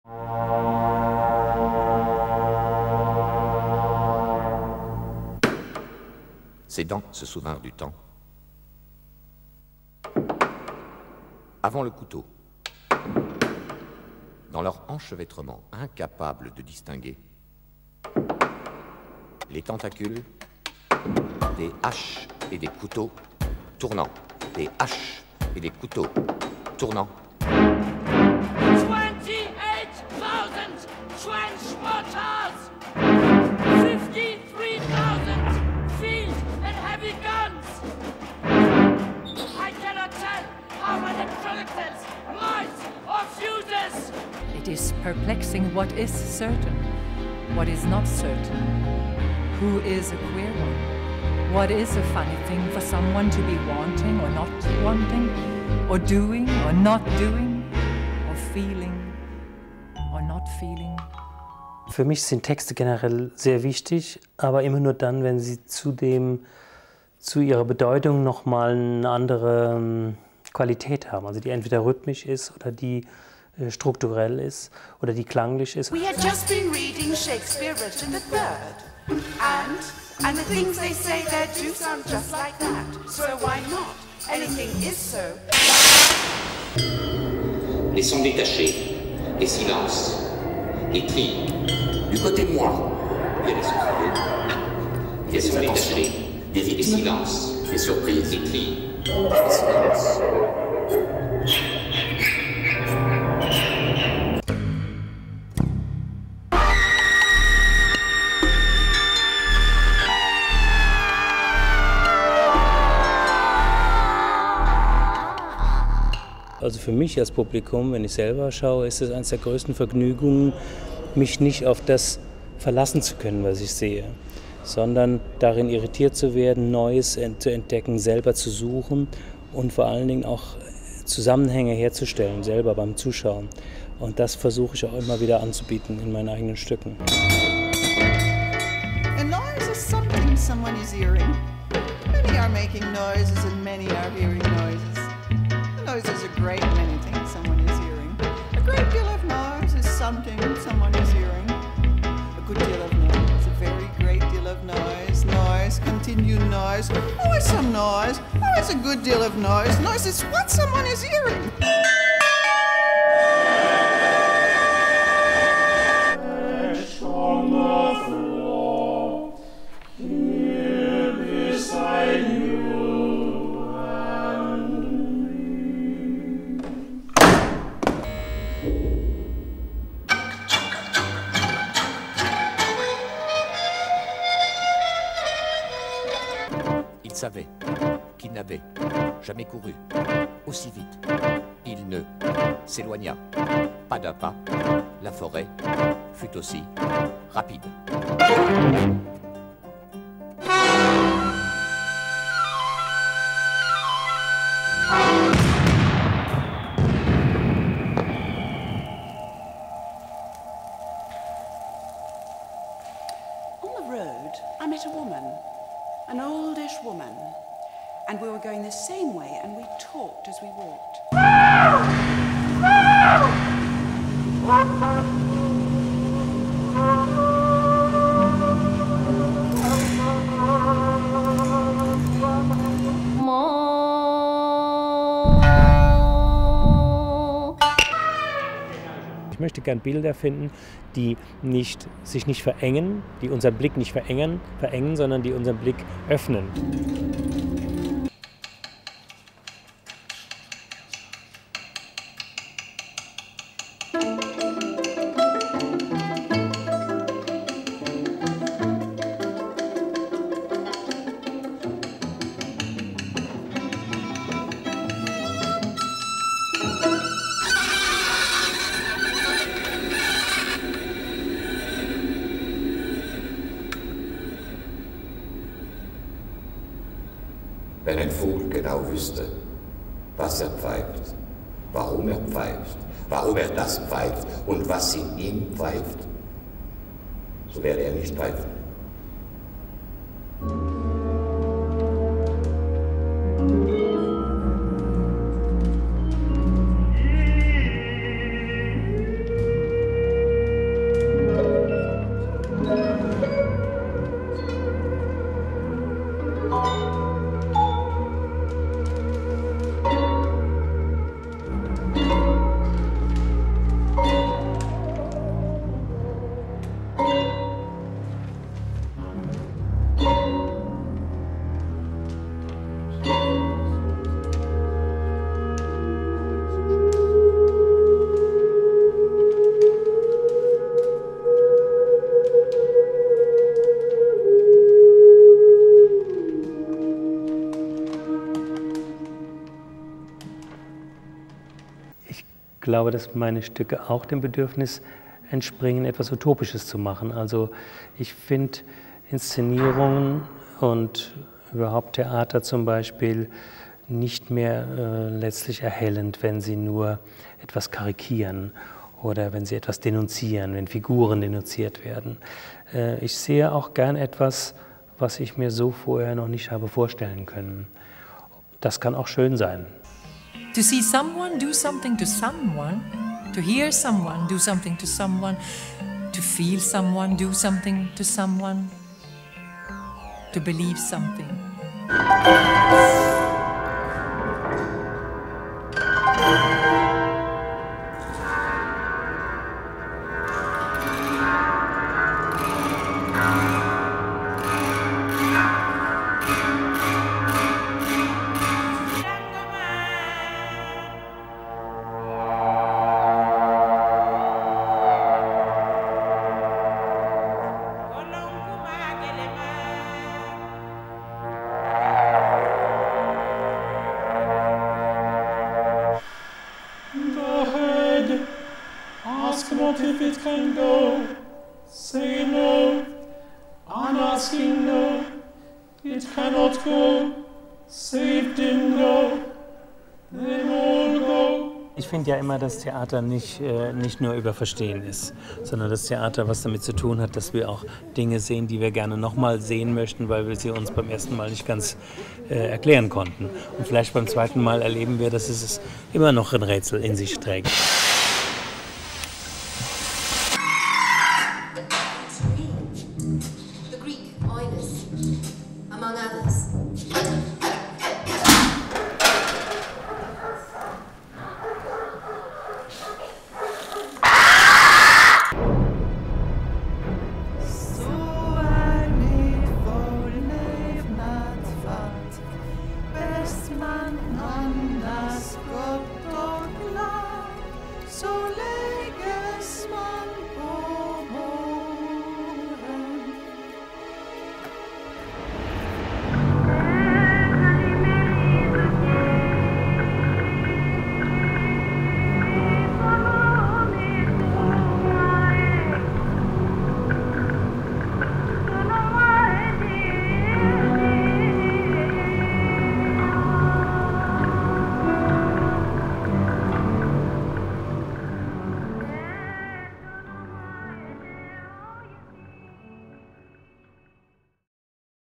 Heiner Goebbels winner of the 2012 International Ibsen Award
Heiner Goebbels is the winner of the 2012 International Ibsen Award. In this video you can hear him talk about his work and see samplers from his performances.

The award ceremony takes place as the finale of the International Ibsen Festival at the National Theatre in Oslo, September 2012.

This video is compiled by Manfred Scheyko.